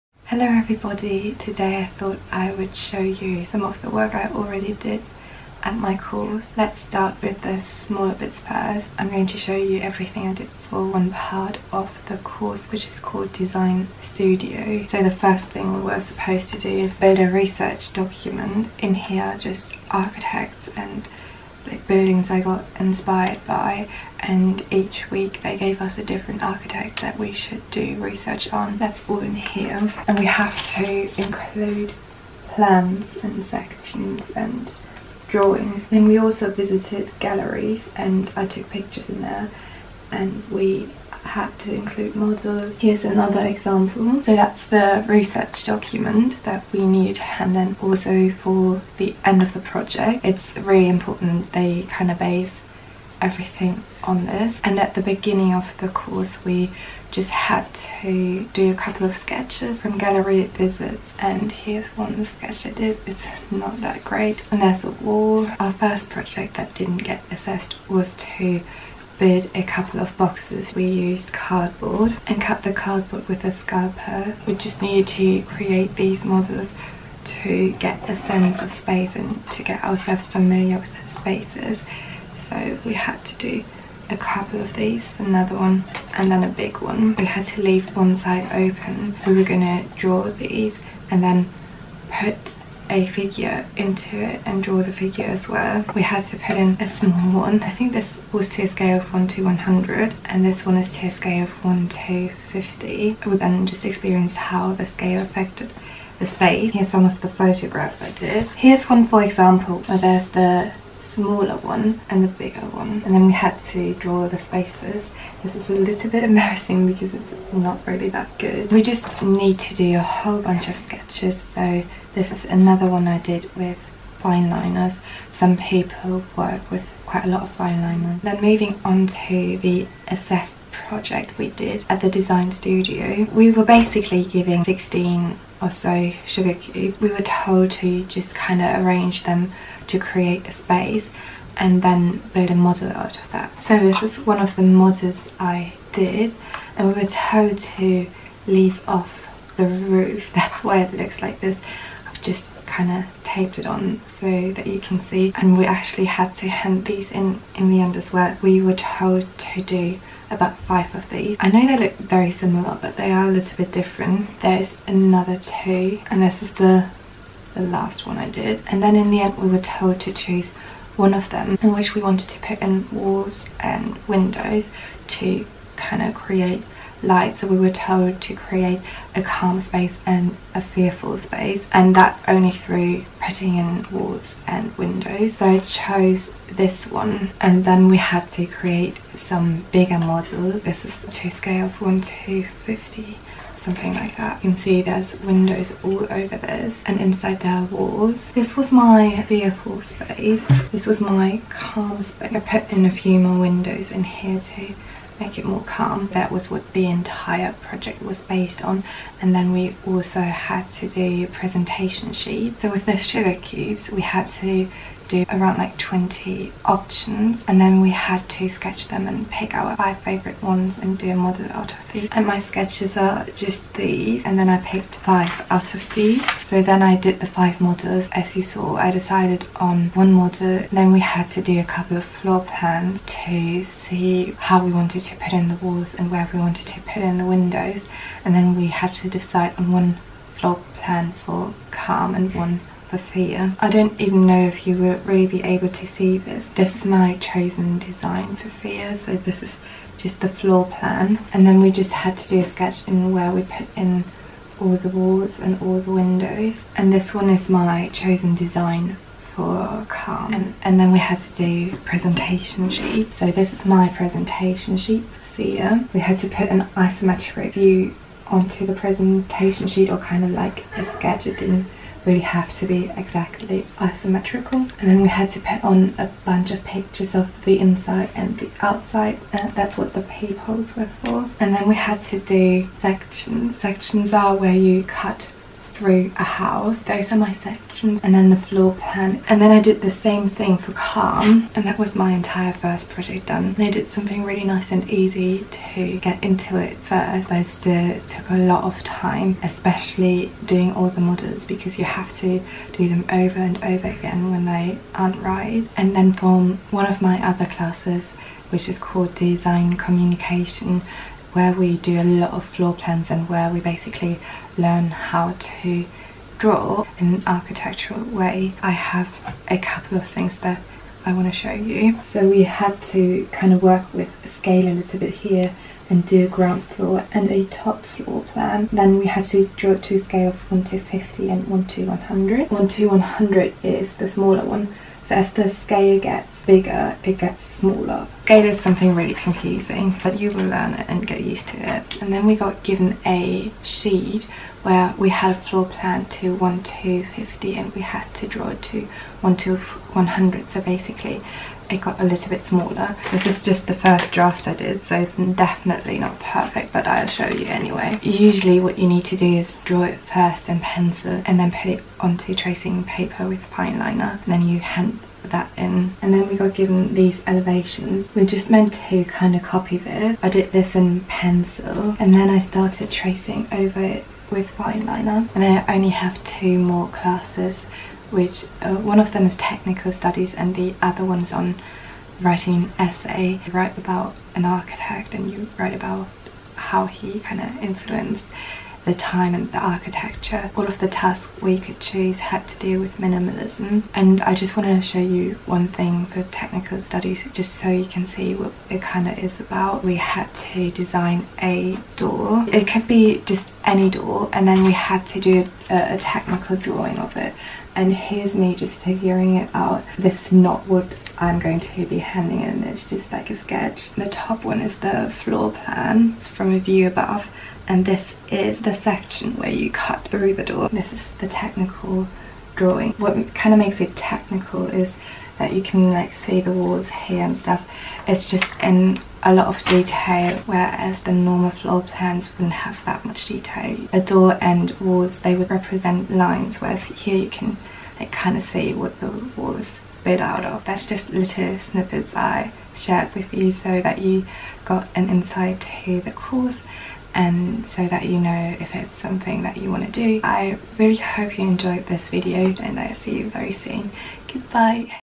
What it’s like to study Interior Design
I am showing you the assignments I get in my Interior design course and the work I did.